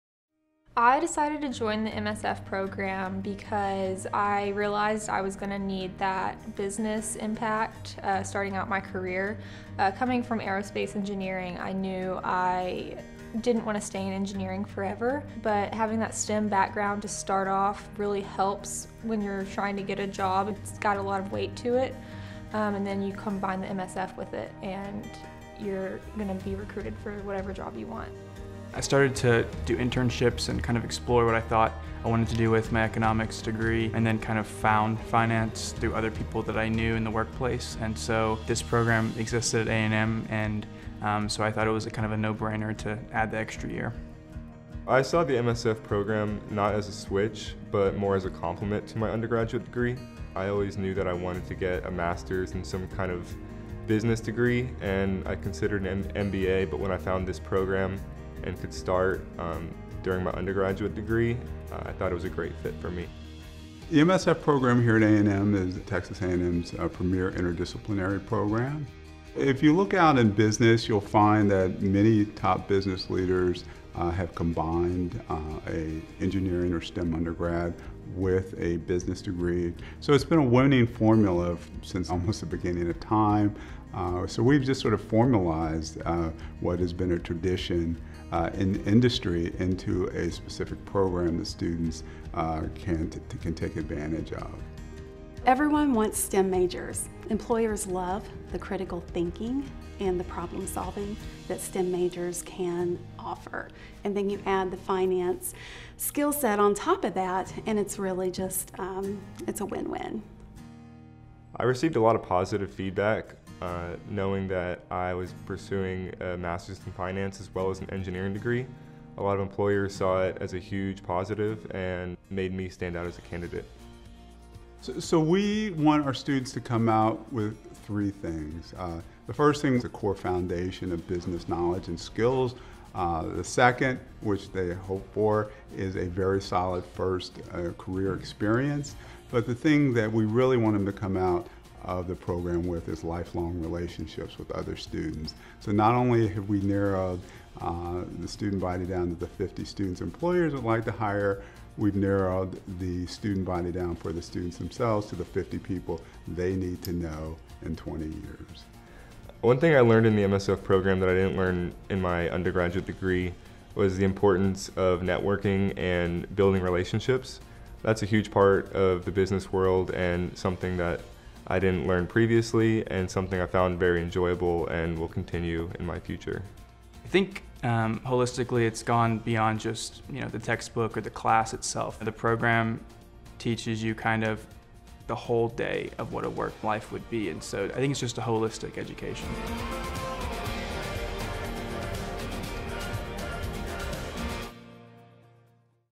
MS Finance at Texas A&M's Mays Business School (2020)
Stem to Stocks. Pair your STEM undergrad with Mays' Master of Science in Finance degree to enter the finance field. Explore more